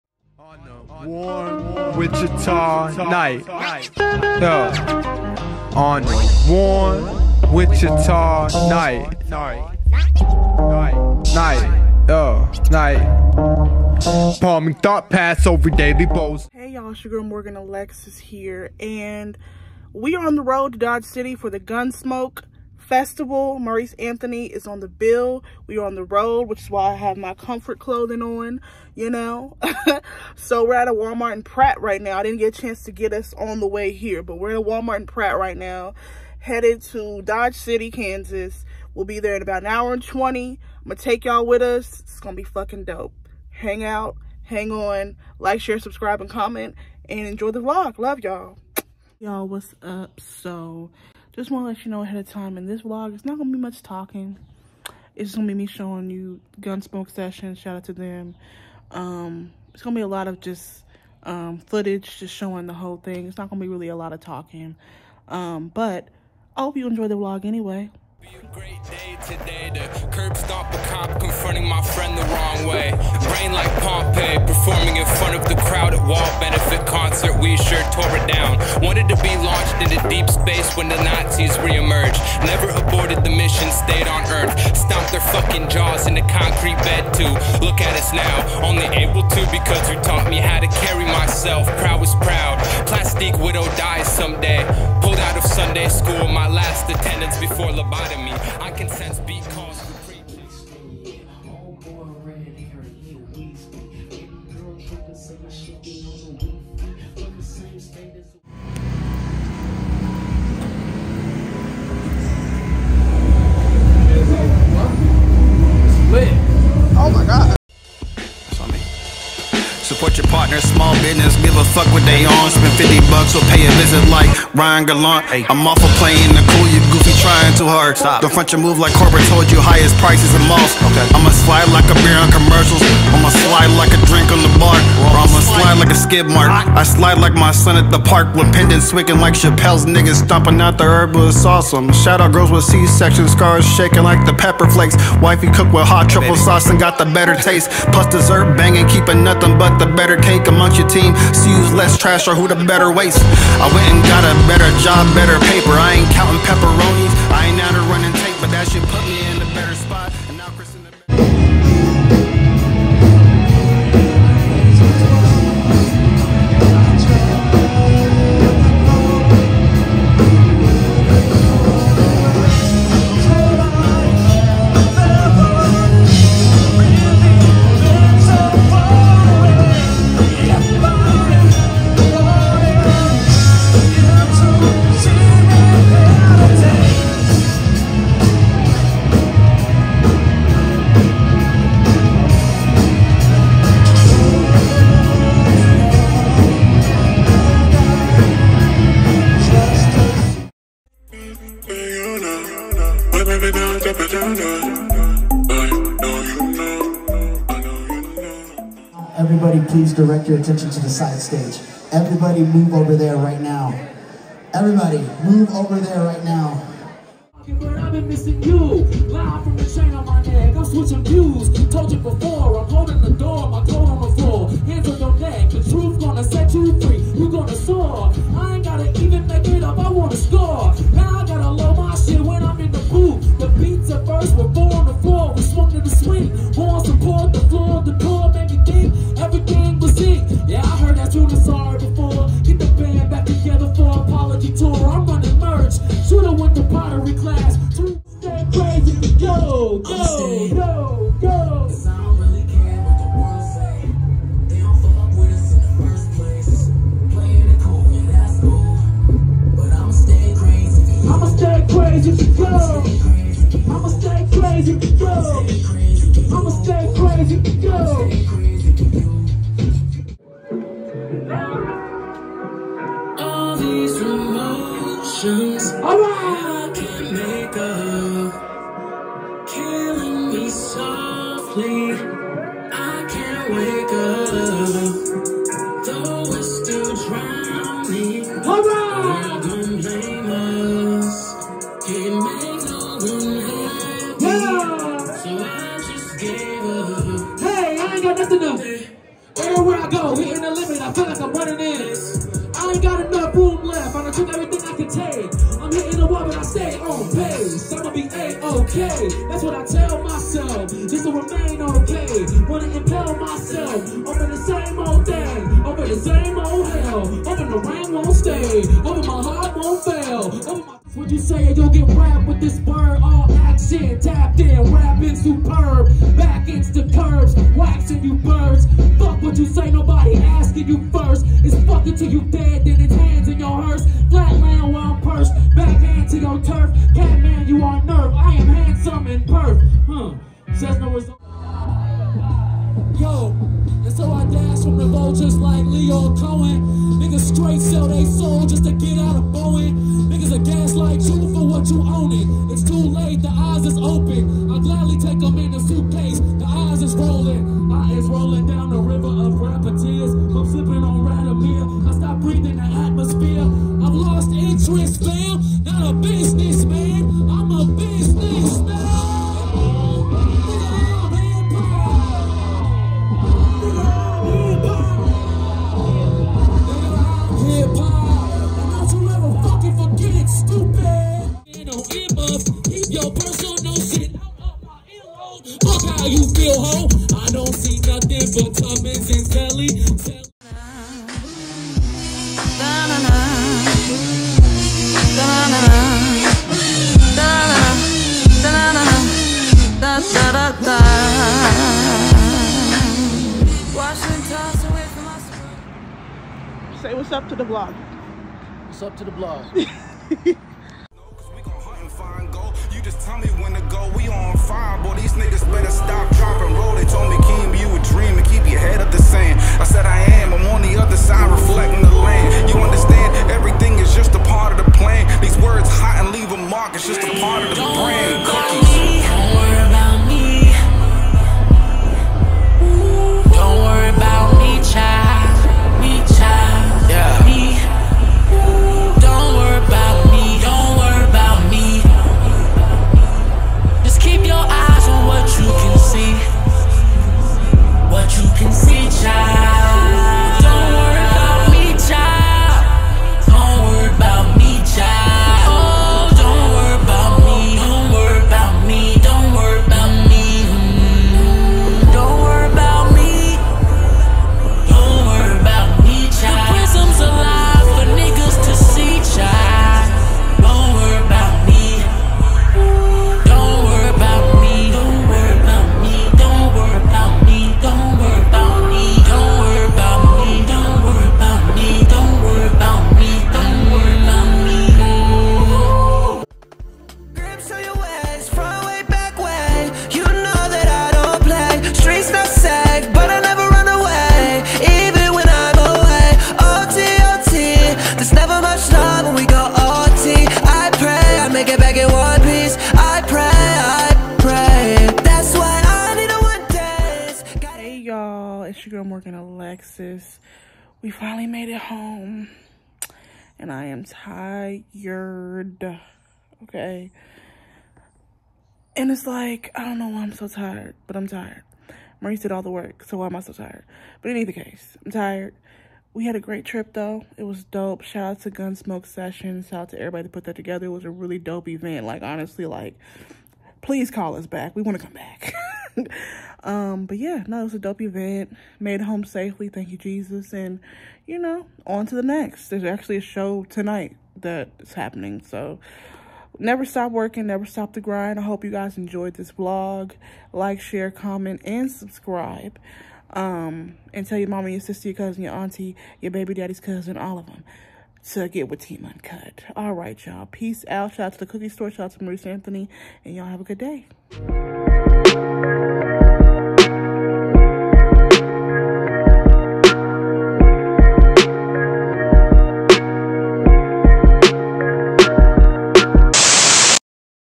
Uncut Vlogs-Gunsmoke Sessions (Dodge City, Ks)
In this vlog we take you with us to Dodge City, Ks for their gunsmoke sessions festival with Marrice Anthony performing!